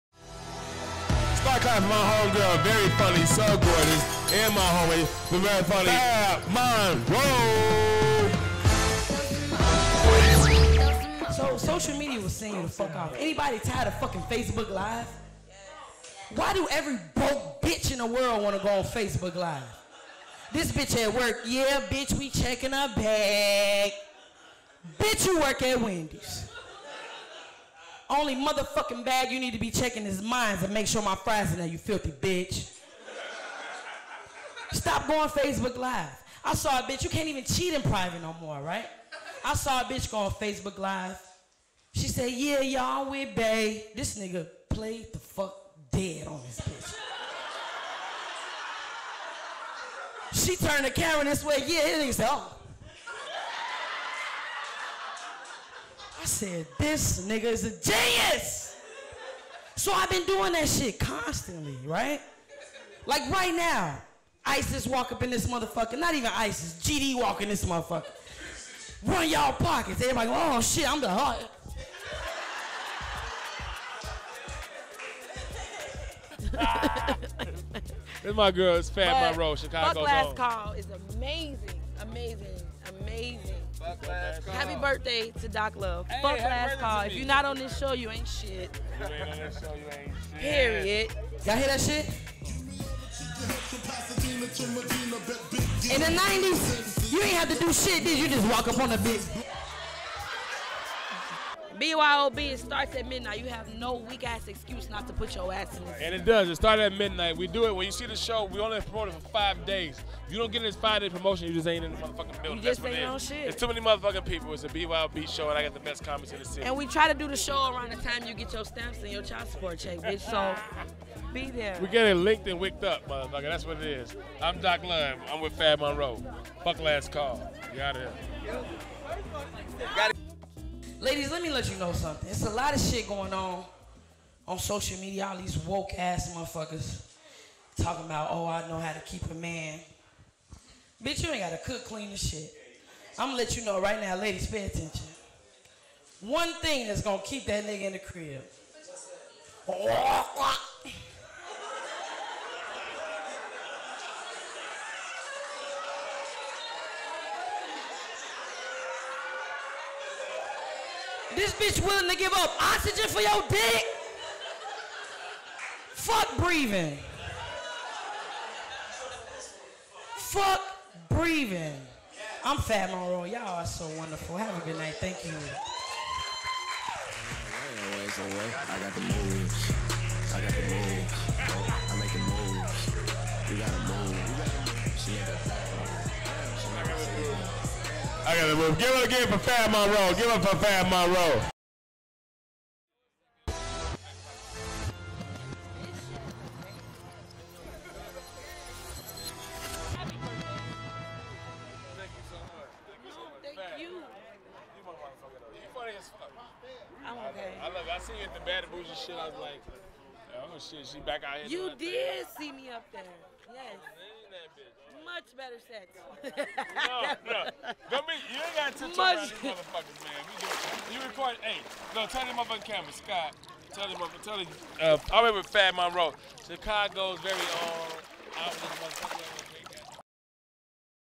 Don't Trust Your Man With This Comic ( Fab Monroe )
Done Waitin presents Fab Monroe.  F*** Last Call is a reoccurring comedy showcase produced by Doc Love

Video by:  CS Media Group
Camera Operator: Scott Simonsen

If you have a question or comment, then we invite you to comment on this video.